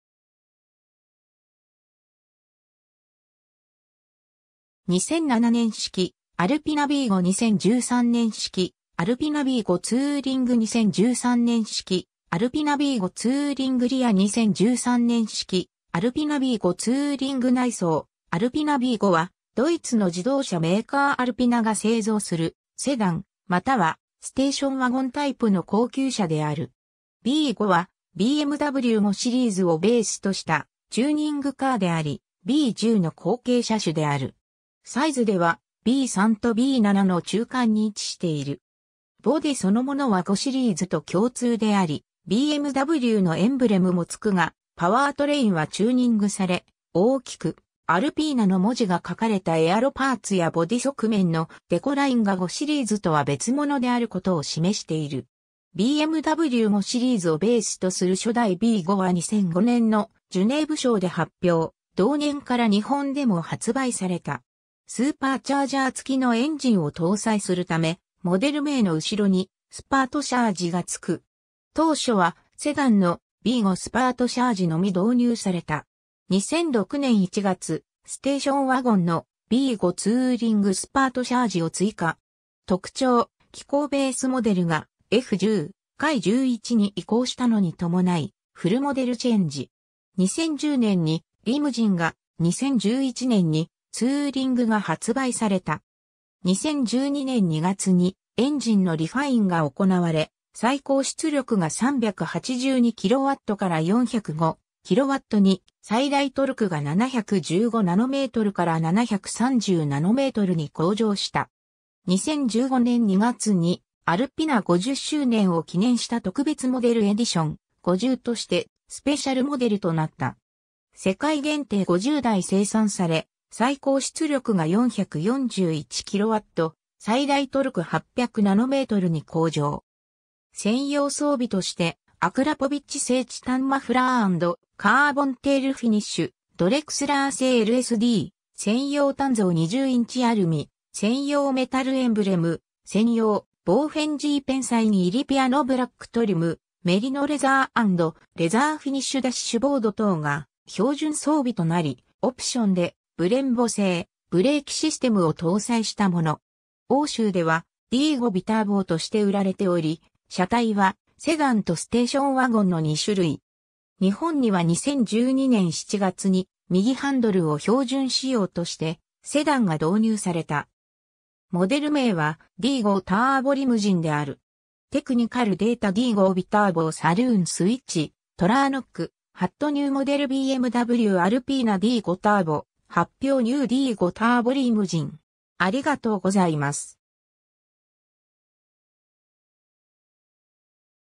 アルピナ・B5
2007年式アルピナB5(E60) 2013年式アルピナB5ツーリング(F10LCI後) 2013年式アルピナB5ツーリングリア(F10LCI後) 2013年式アルピナB5ツーリング内装(F10LCI後) アルピナ・B5（ビーファイブ）は、ドイツの自動車メーカーアルピナが製造するセダンまたはステーションワゴンタイプの高級車である。
 B5はBMW・5シリーズをベースとしたチューニングカーであり、B10の後継車種である。
サイズではB3とB7の中間に位置している。
ボディそのものは5シリーズと共通であり、BMWのエンブレムも付くが、パワートレインはチューニングされ、大きく「ALPINA」の文字が書かれたエアロパーツやボディ側面のデコラインが5シリーズとは別物であることを示している。
 BMW・5シリーズ（E60/E61）をベースとする初代B5は2005年のジュネーブショーで発表、同年から日本でも発売された。
スーパーチャージャー付きのエンジンを搭載するため、モデル名の後ろに「Supercharge」が付く。
当初はセダンの「B5 Supercharge」のみ導入された。
 2006年1月、ステーションワゴンの「B5 Touring Supercharge」を追加。
 特徴・機構 ベースモデルがF10/F11に移行したのに伴いフルモデルチェンジ。
2010年にリムジン（セダン）が、2011年にツーリング（ワゴン）が発売された。
 2012年2月にエンジンのリファインが行われ、最高出力が382kw(520馬力)から405kw(550馬力)に、最大トルクが715Nm(72.9kgm)から730Nm(74.4kgm)に向上した。
 2015年2月にアルピナ５０周年を記念した特別モデルＥＤＩＴＩＯＮ５０としてスペシャルモデルとなった。
世界限定５０台生産され、最高出力が441Kw(600馬力)、最大トルク800Nm(81.6kgm)に向上。
専用装備として、アクラポビッチ製チタンマフラー＆カーボンテールフィニッシュ、ドレクスラー製ＬＳＤ、専用鍛造20インチアルミ、専用メタルエンブレム（シート＆キーホルダー）,専用ボーフェンジーペンサイン入りピアノブラックトリム、メリノレザー＆レザーフィニッシュダッシュボード等が標準装備となり、オプションでブレンボ製ブレーキシステム（ディスク径395mm)ＡＬＰＩＮＡネーム入りブルー塗装やヘリテイジシートなどの専用オプションも用意される。 特徴・機構 D5 D5は直列6気筒の3Lターボディーゼルエンジン（アルピナ・N57エンジン）を搭載したもの。
欧州ではD5 BITURBOとして売られており、車体はセダンとステーションワゴンの二種類。
 日本には2012年7月に右ハンドルを標準仕様としてセダンが導入された。
モデル名はD5 Turbo Limousineである。
 ^ TECHNICAL DATA D5 BITURBO Saloon SWITCH-TRONIC ^ ニューモデル BMW ALPINA D5 Turbo発表 ^ New D5 Turbo Limousine